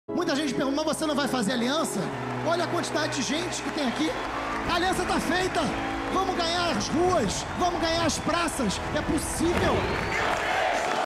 Nossa aliança | Horário Eleitoral
A nossa aliança está feita! É, sempre foi e sempre será com as pessoas!

Veja mais um propaganda do horário eleitoral que foi na TV.